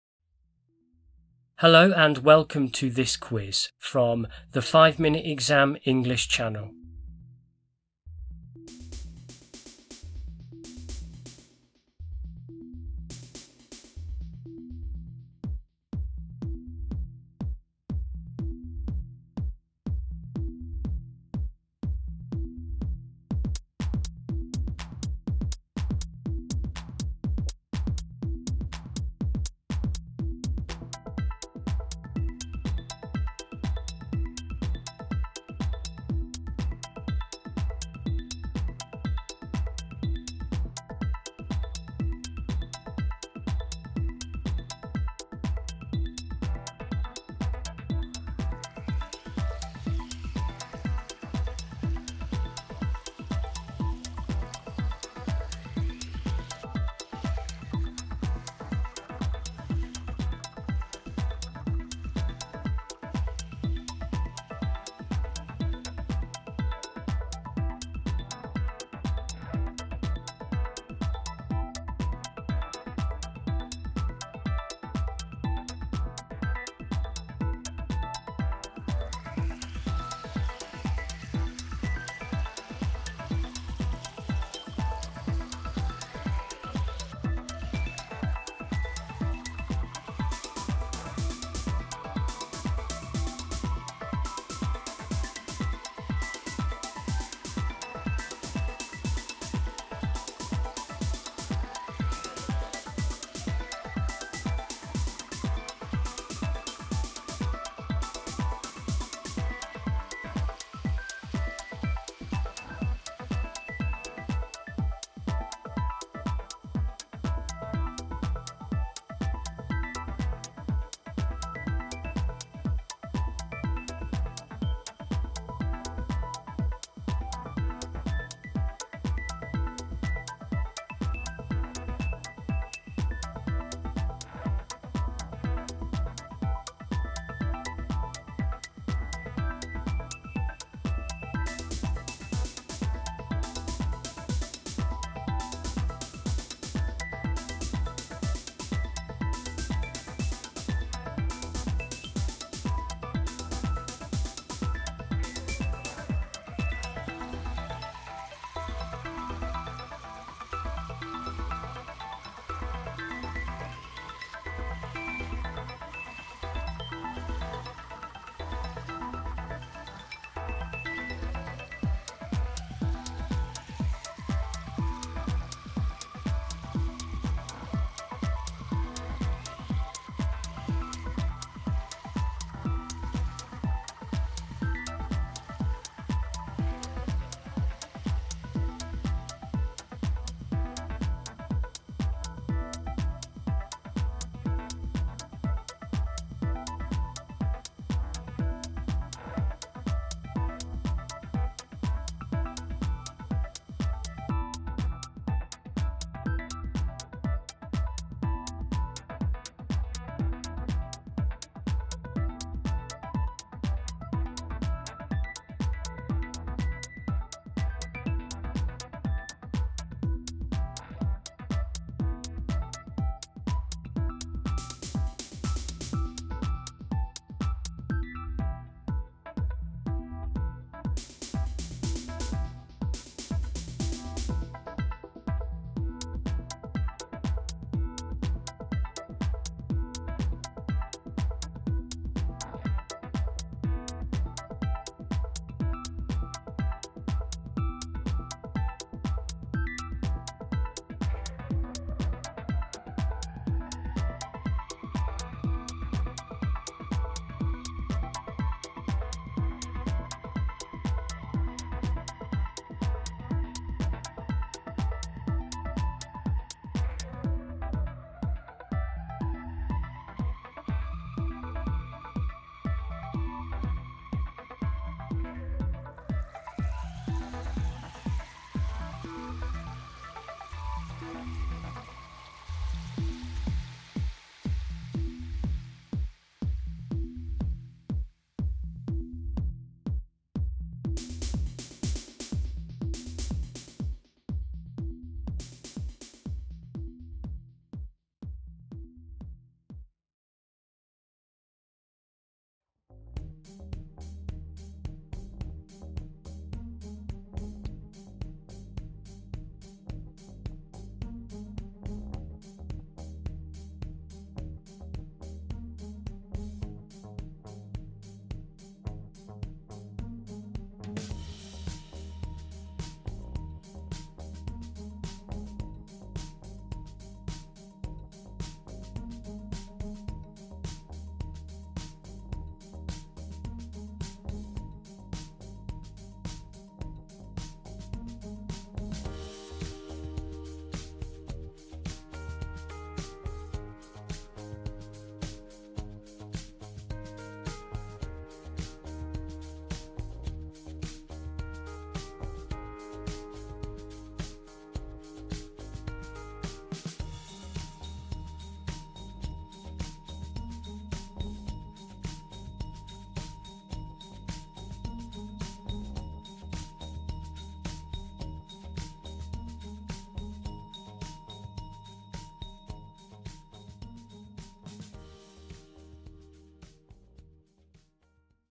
American vs British English Quiz | A Vocabulary Quiz for English Learners
How well do you the differences between British and American varieties of English? Try this quiz and find out!

This quiz features 15 multiple-choice questions that will test your knowledge of vocabulary and spelling.

There are lots more quizzes for English students on this channel.

US and UK flags on thumbnail from pngtree.com